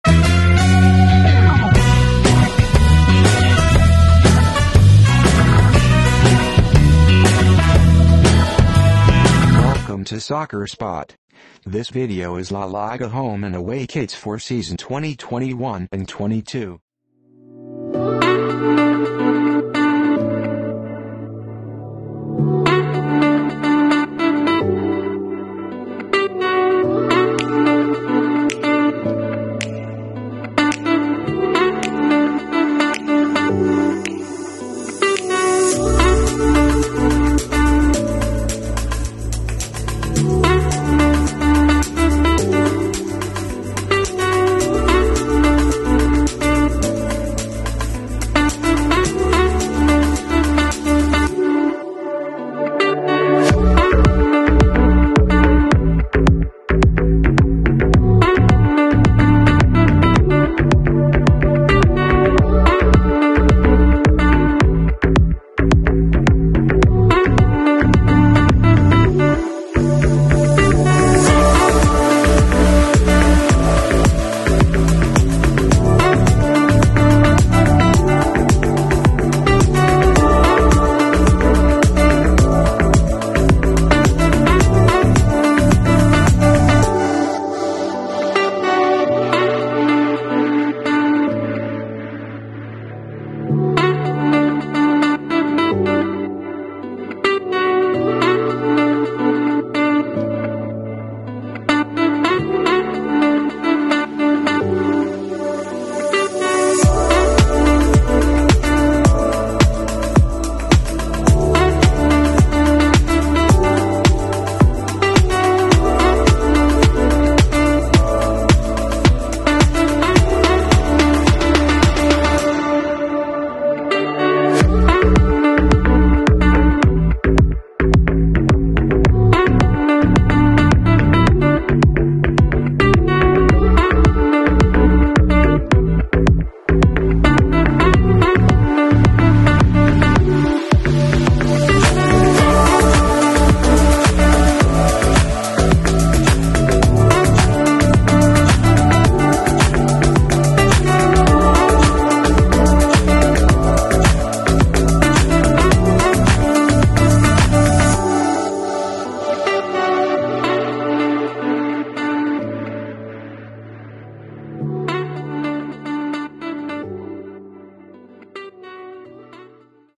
La Liga Home And Away Kits For Season 2021-22 | Barcelona, Real Madrid And Many More Club
Kit2022 | HomeAwayKits2022 | Fifa World Cup | La-liga | uefa | Fifa | Champions league | Premier League | Real Madrid | Barcelona | Juventus | Top Goals | Top Players